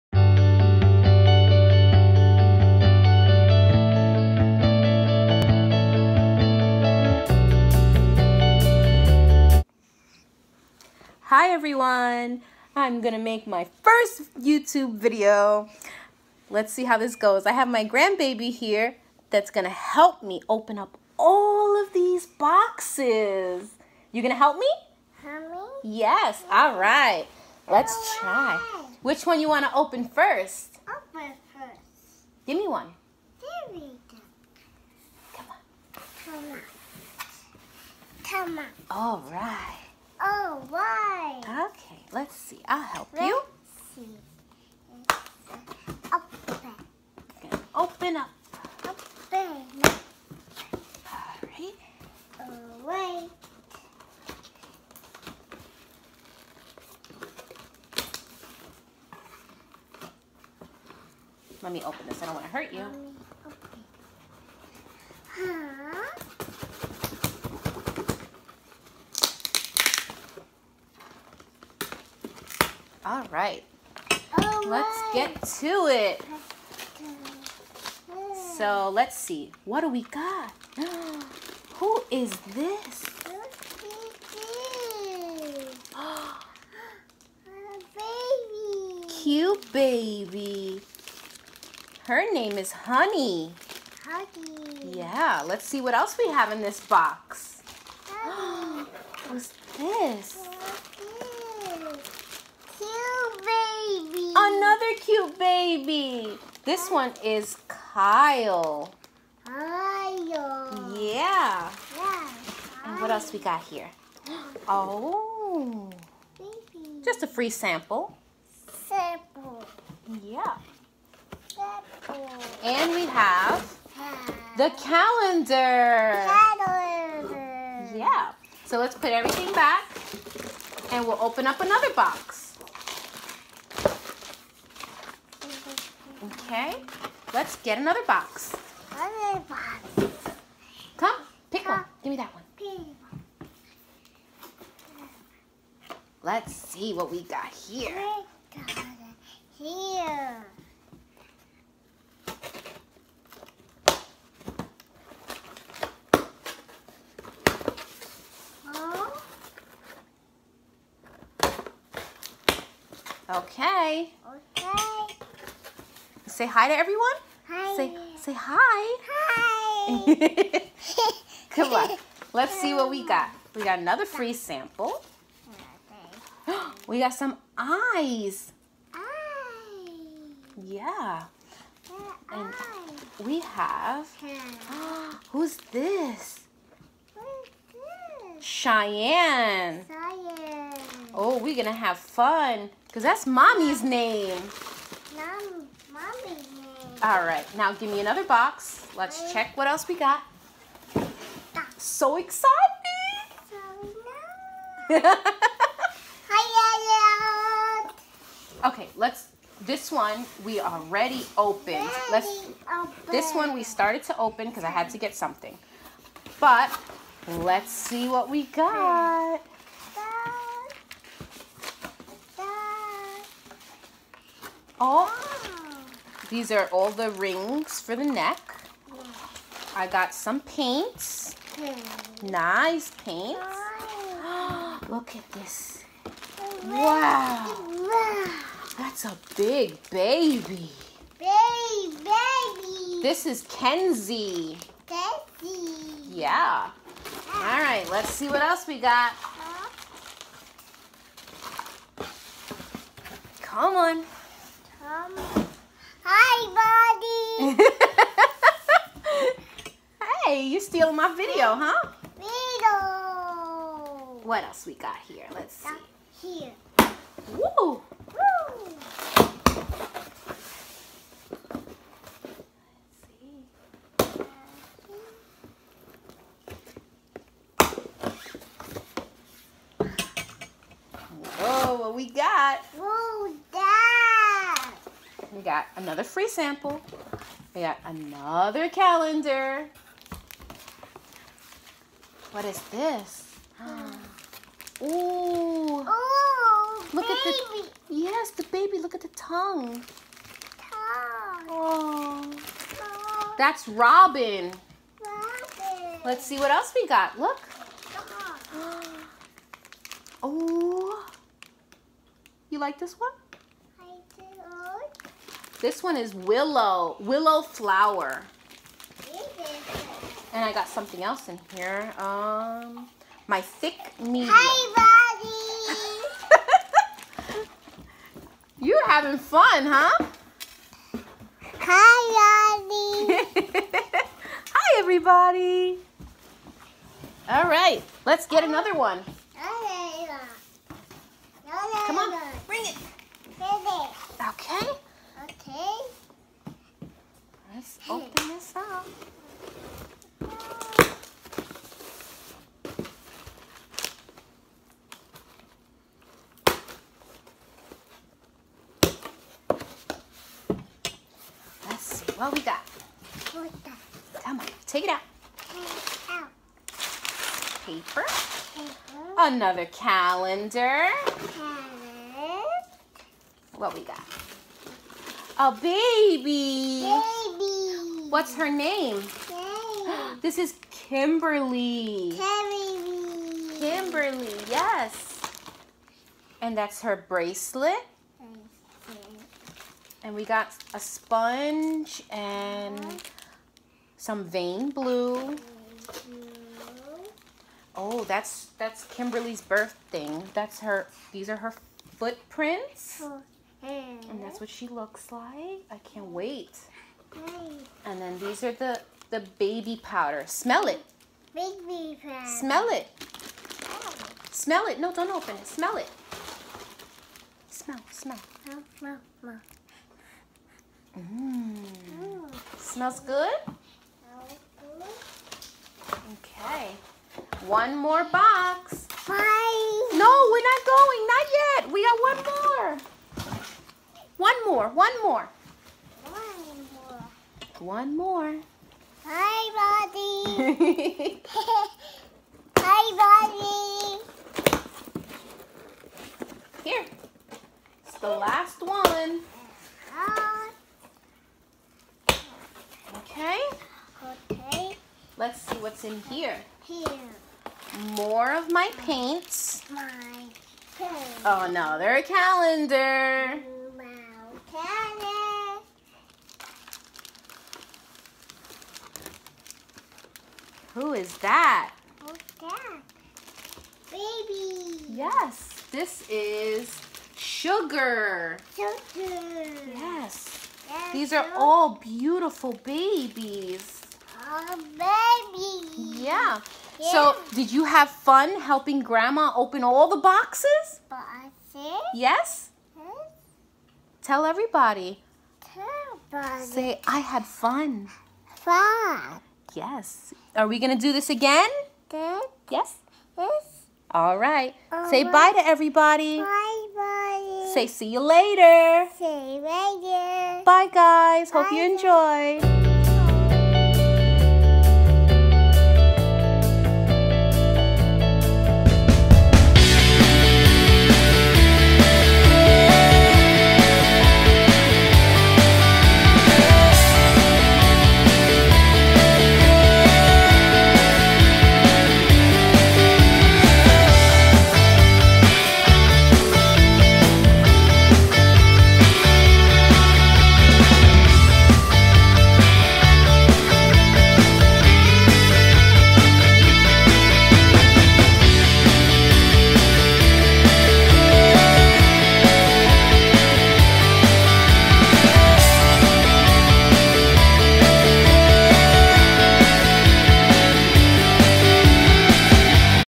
Bountiful Baby Unboxing
Here's an unboxing of a haul of Bountiful Babies.
1) Honey
2) Kyle
3) Shyann
4) Kenzie
5) Robin
6) Willow Flower
7) Kimberly
8) Sugar